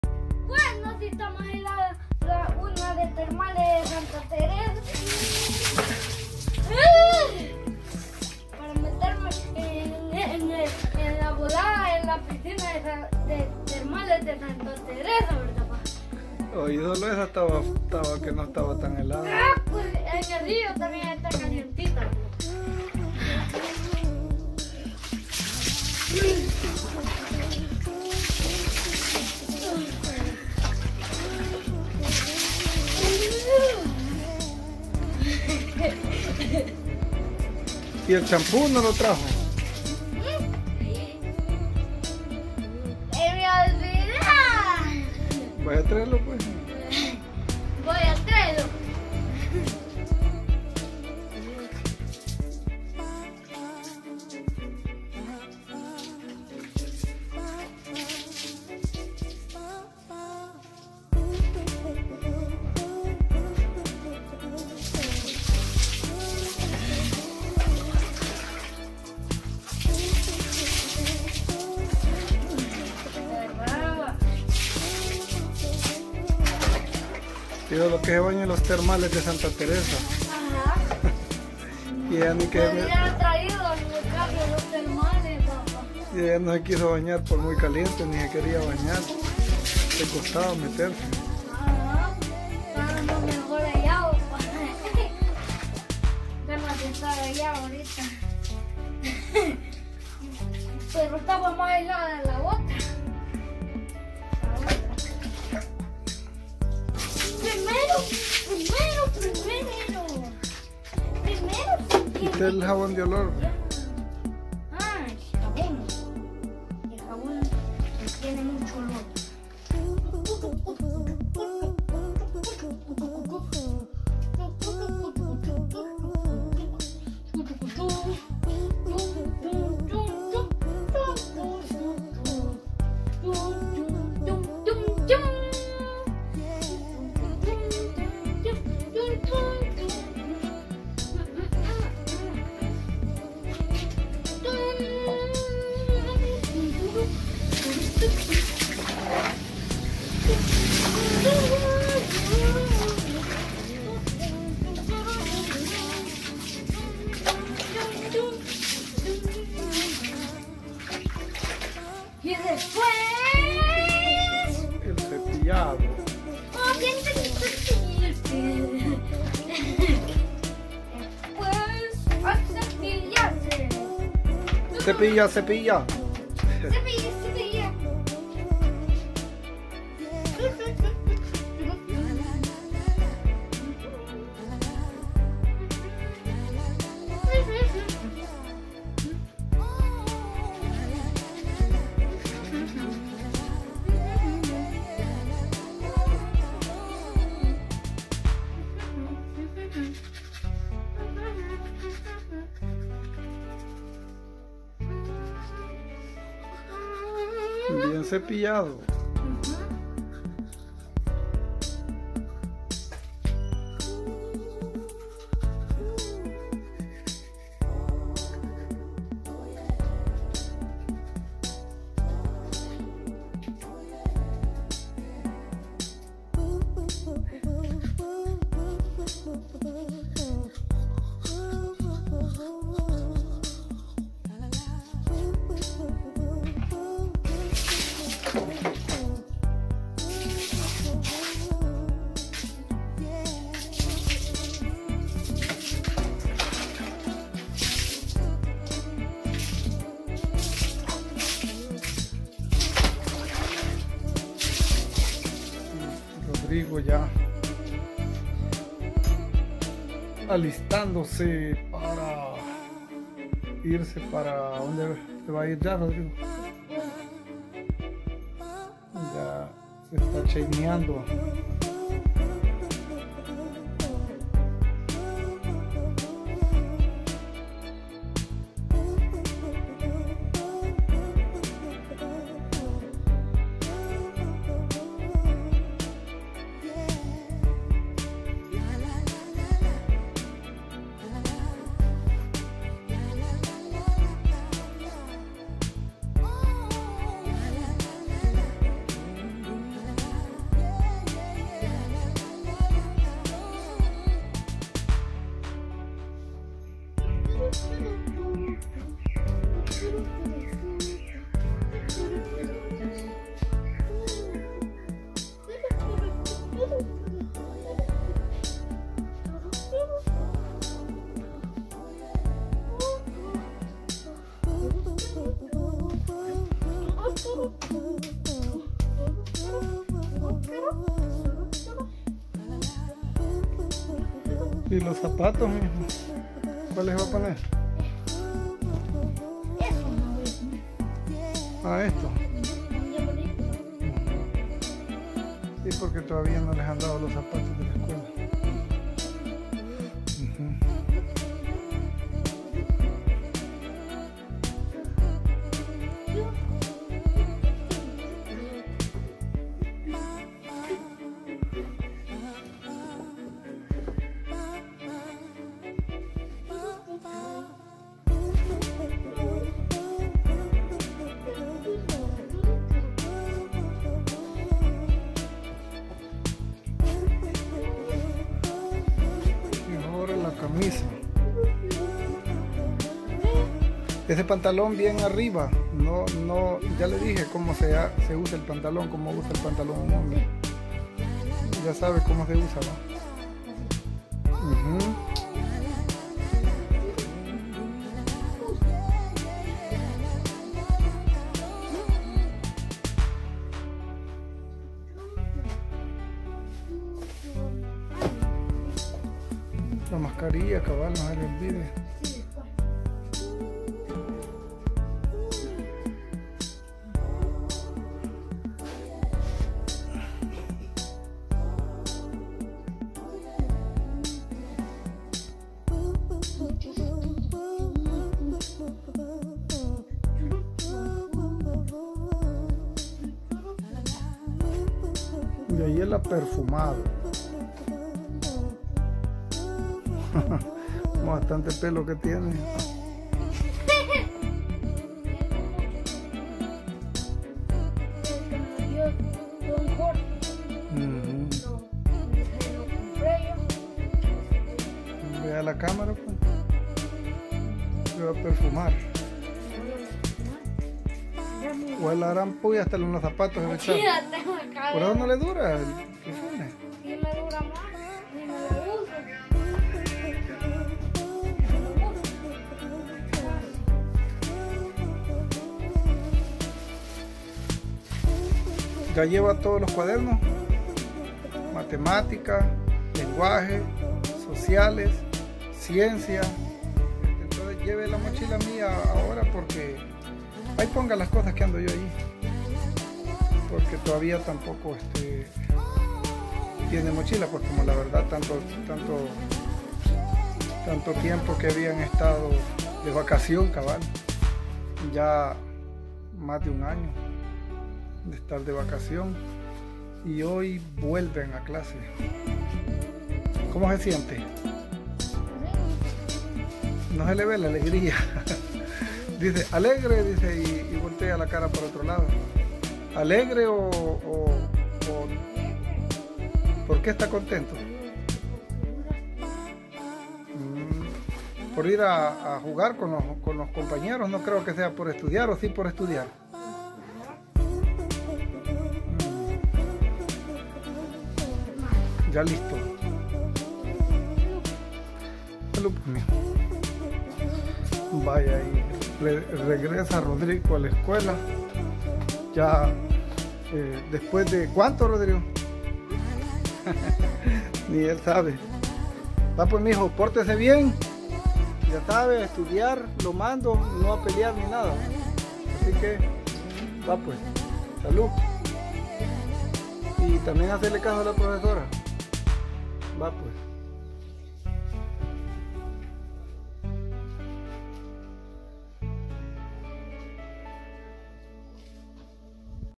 MIREN PORQUE LA ALEGRIA DE RODRIGO.
las clases inician en el Salvador semi presenciales y Rodrigo tambien vuelve a la escuela en el complejo educativo Cantón Barahona